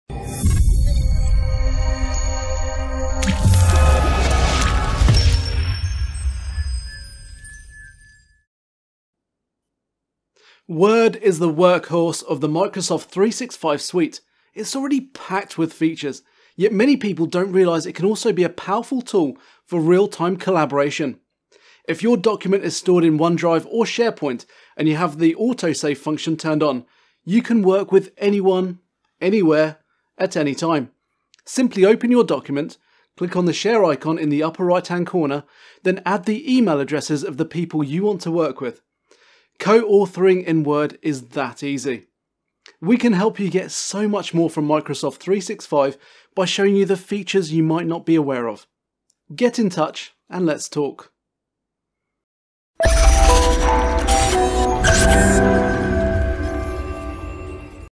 Collaborating in real time with Word
We all love Word, and have used it for many, many years.

But did you know the Word of today can do so many more things, compared to the Word of yesterday.

Such as collaborating on a document in real time. Here’s how…